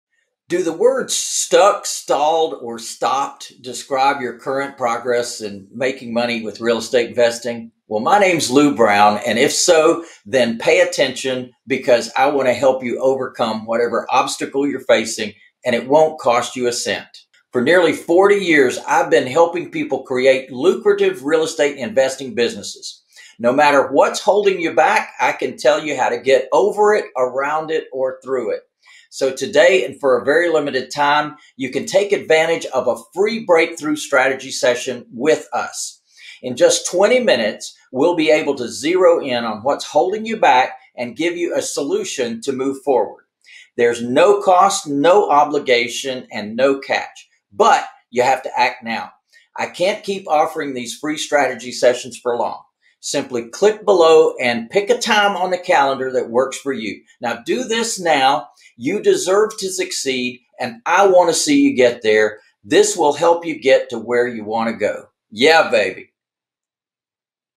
Free Breakthrough Strategy Invitation
* Money Making Secret #1:
Getting Motivated Sellers to Call You! Stop calling unmotivated sellers and getting rejected more times than a geek for the high school prom! I'll show you how for over 28 years, I've been able to get deals brought to me and have motivated sellers hunting me down to give me their house.

* Money Making Secret #2:
The Art of Structuring Deals! I've been doing this for 40 years, and I love to share how I get sellers to give me the house, a check, and even a jet ski or two.

* Money Making Secret #3:
Negotiating Secrets Revealed! I love to negotiate, ask Matt, one of my hand-picked coaches. We recently went to an estate sale, and he witnessed me structuring a deal to buy the house and contents right there on the spot.

* Money Making Secret #4:
Trusts Simplified! A little confused on this entire Trust thing? Why do it? How to do "Subject-To" safely? I'm going to show you my new fundamentals of Trusts training that will simplify the concept of Trusts so much that a 9-year-old child can understand it.

* Money Making Secret #5:
Develop a Plan for Success in Real Estate. The wealthiest man ever to walk the earth left clues for us on how he amassed his great wealth and kept it.